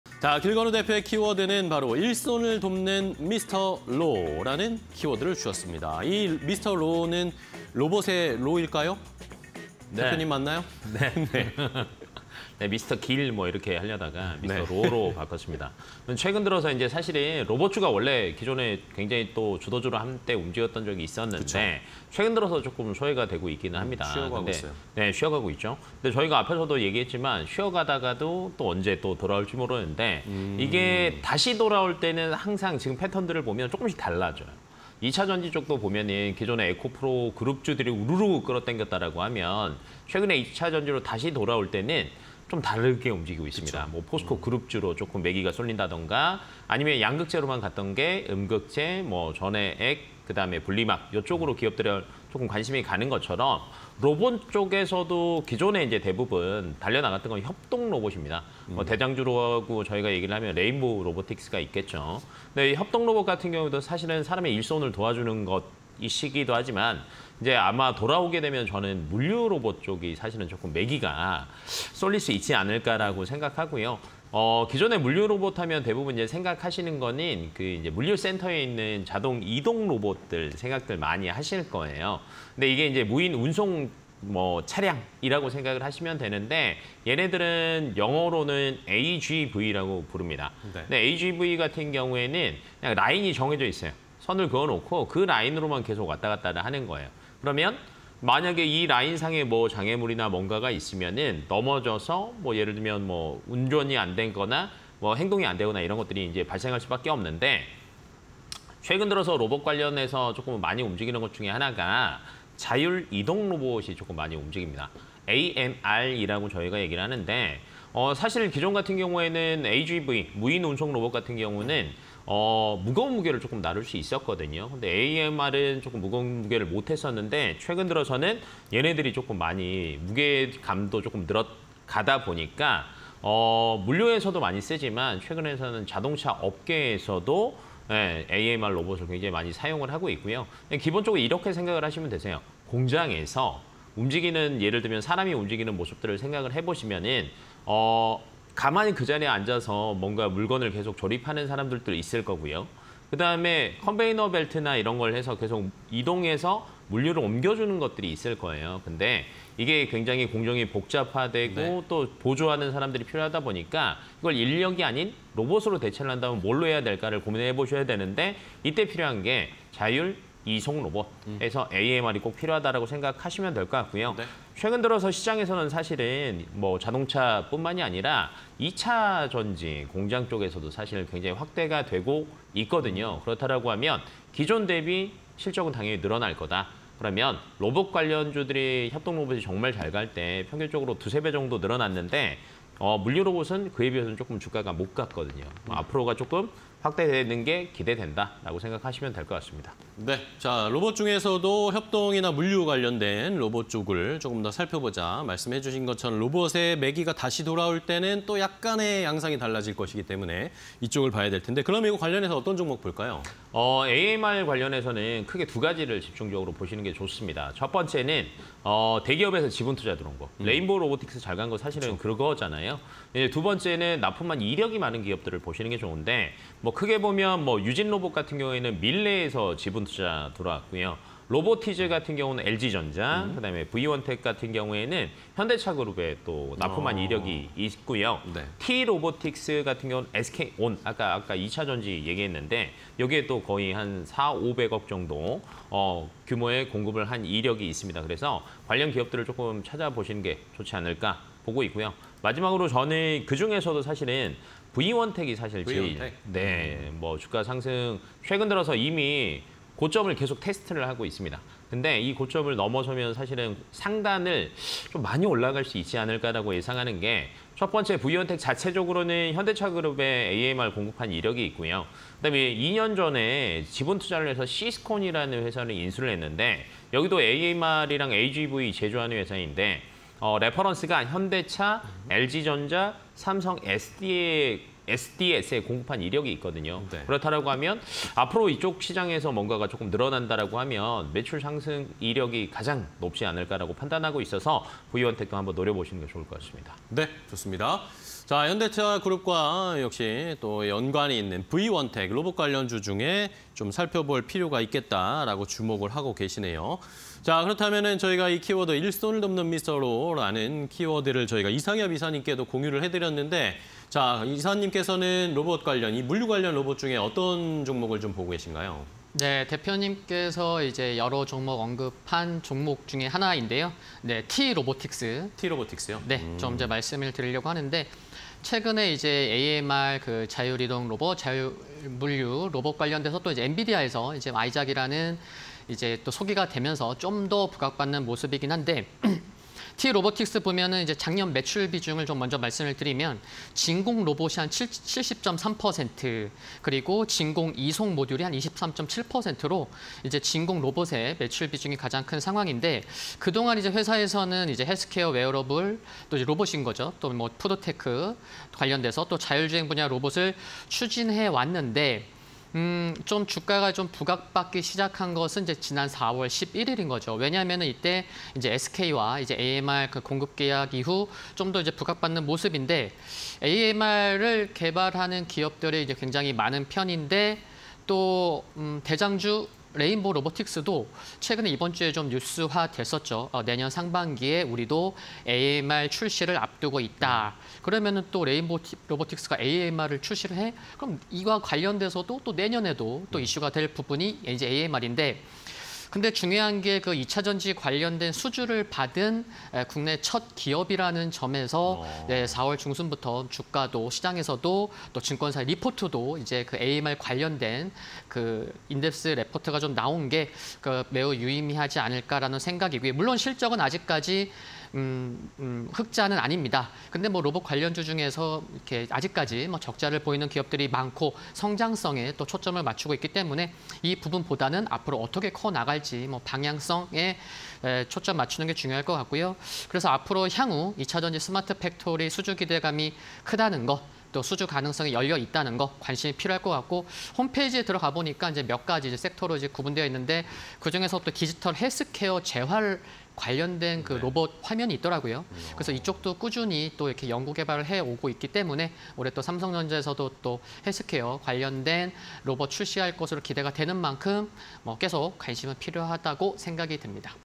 브이원텍(251630), 티로보틱스(117730)... 물류 로봇_키워드 배틀 (20230609)
물류 로봇
- 자율주행 가능 AMR 사용처 확대 중
- 구독형 서비스 도입에 따른 AMR 시장 확대
- 협동로봇 상승력에 비해 아직은 여유

브이원텍(251630)
- 2차전지ㆍ디스플레이 장비 업황 회복 수혜
- 현대차그룹에 AMR 공급 이력
- 2024년 로봇 사업부 매출, 기존 사업부 상회 전망

티로보틱스(117730)
- 지난해 매출 비중: 진공로봇 70%ㆍ진공이송모듈 23%
- 4월, SK와 AMR 공급 계약 이후 부각
- 향후 2차전지 스마트팩토리향 수주 기대